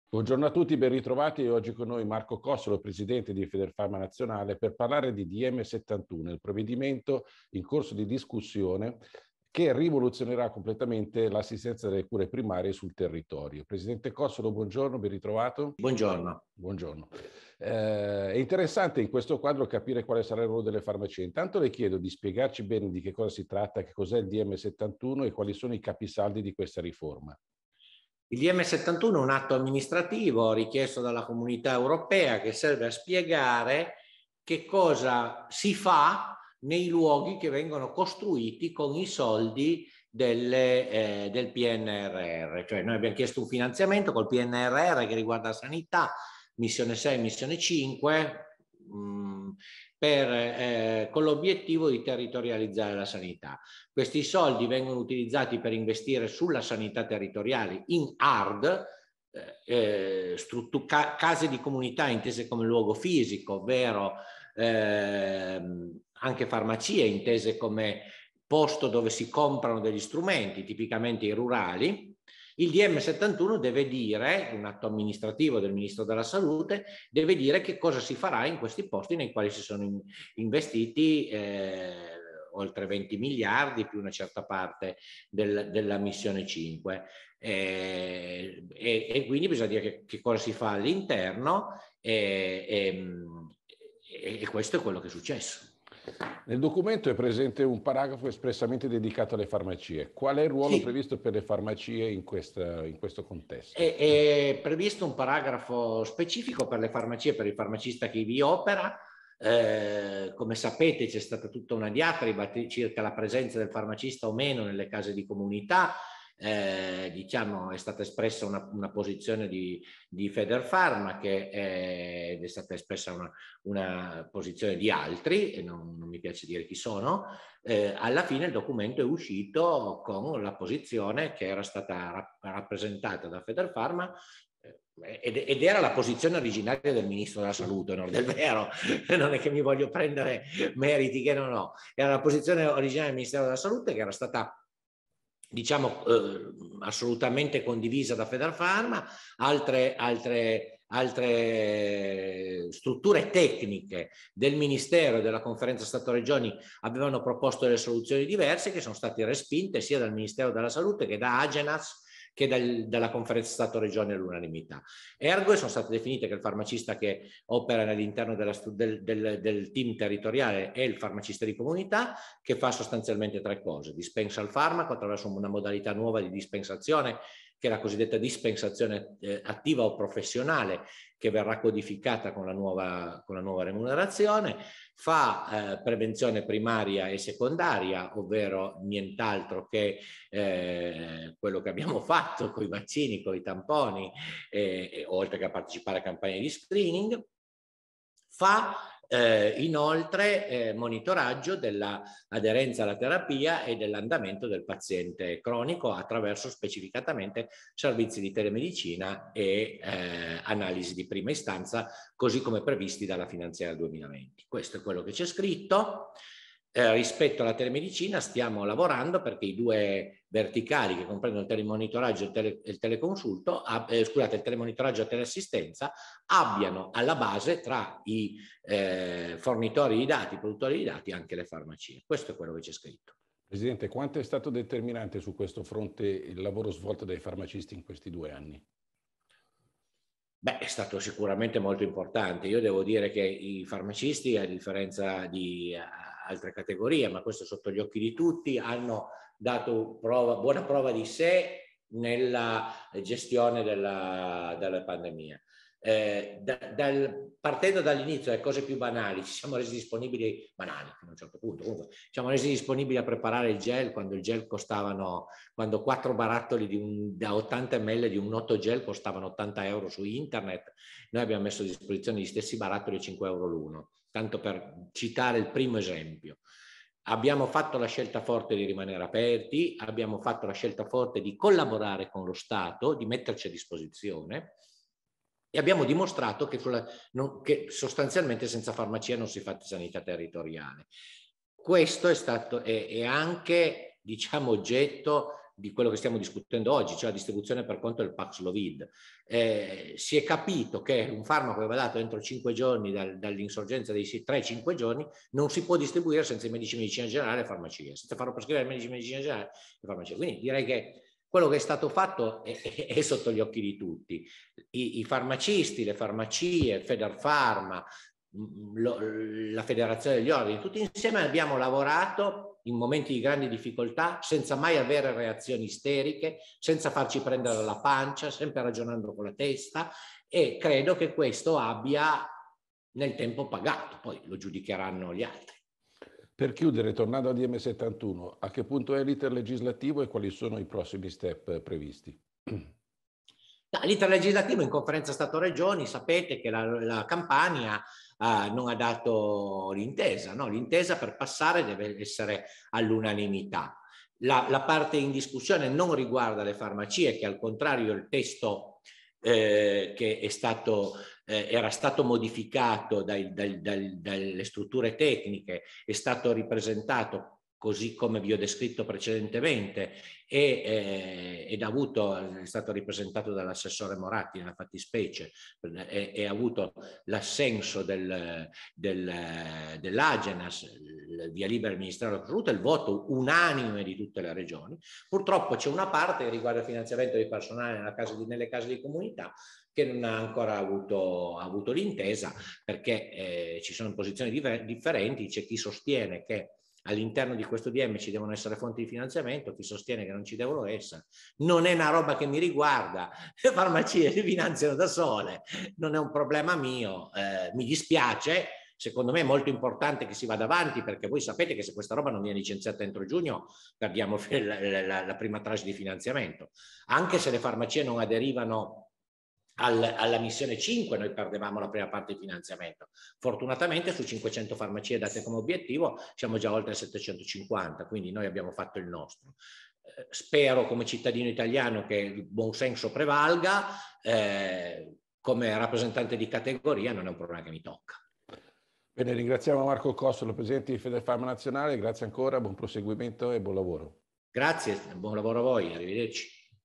Sanità territoriale, la rivoluzione del DM71
Con i fondi del PNRR va ridisegnandosi l’assistenza primaria. Ruolo chiave delle farmacie su tre fronti, come ribadisce Marco Cossolo, presidente di Federfarma: dispensazione del farmaco, prevenzione primaria e secondaria, monitoraggio dell’aderenza alle terapie e del paziente cronico.

di Nicola Miglino